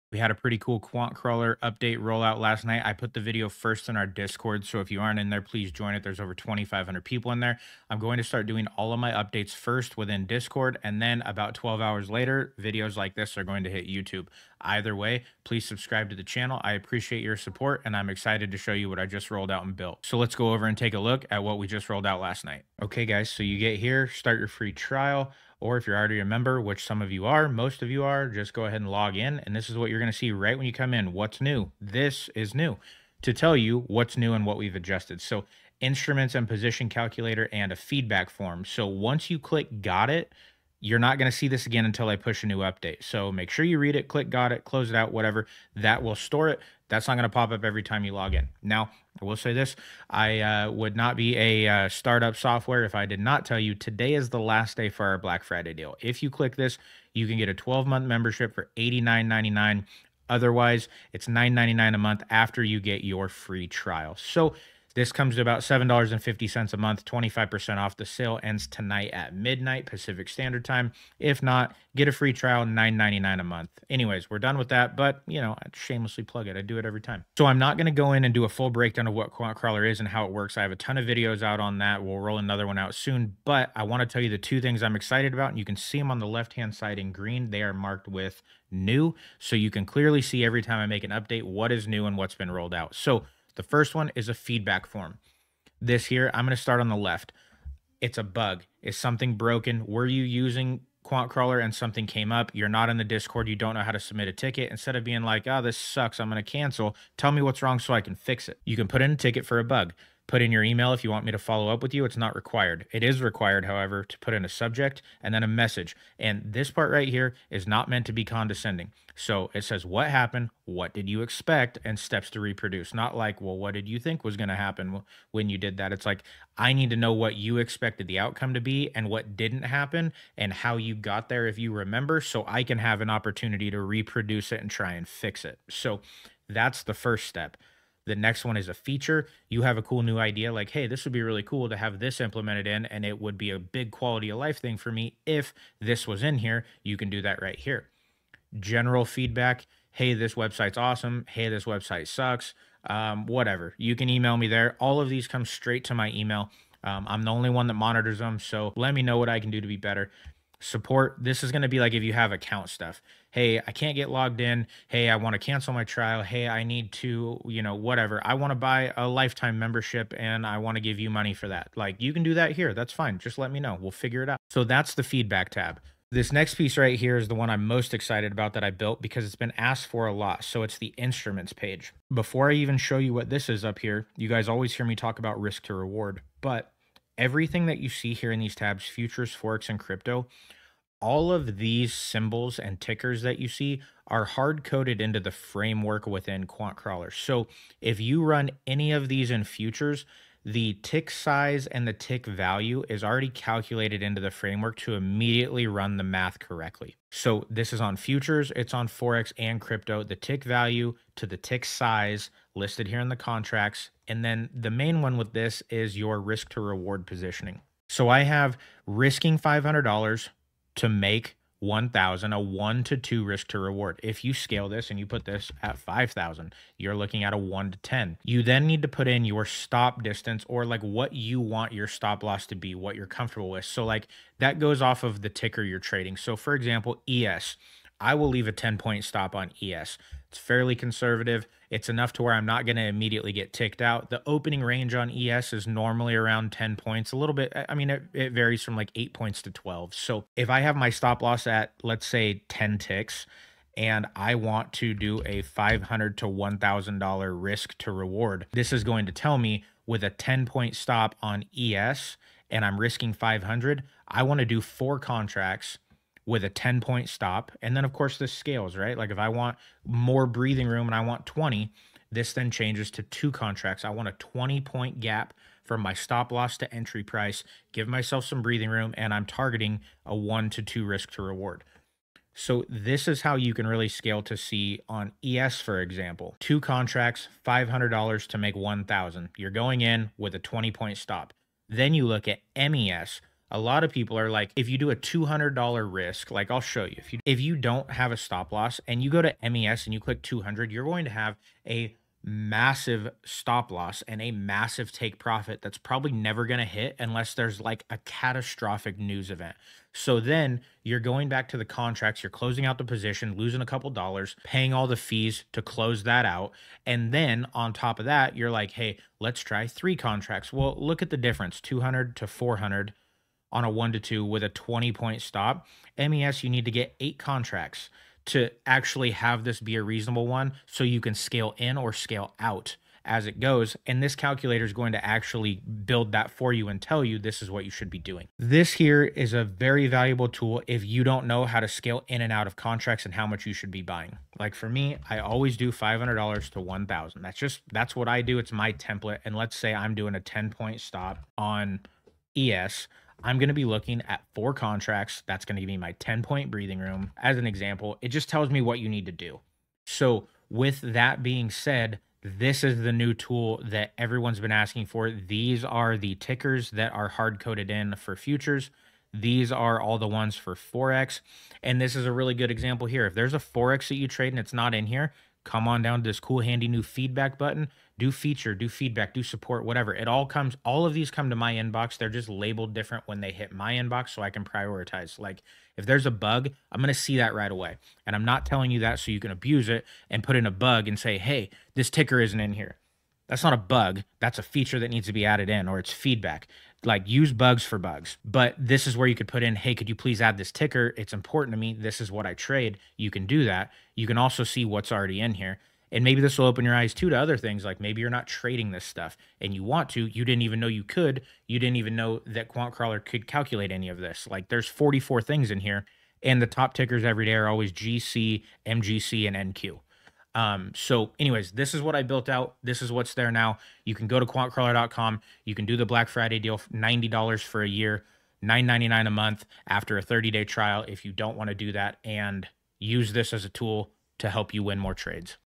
QuantCrawler Just Got a Major Upgrade (Huge New Tools)
This QuantCrawler update adds new features users have been asking for: a feedback system, bug reporting, support requests, and a full instrument + position calculator page. This video walks you through everything that rolled out, how to use it, and why these updates matter for futures traders. If you use GC, NQ, MES, ES, or Forex pairs, these new tools dial in your risk, position size, and workflow.

=== HASHTAGS ===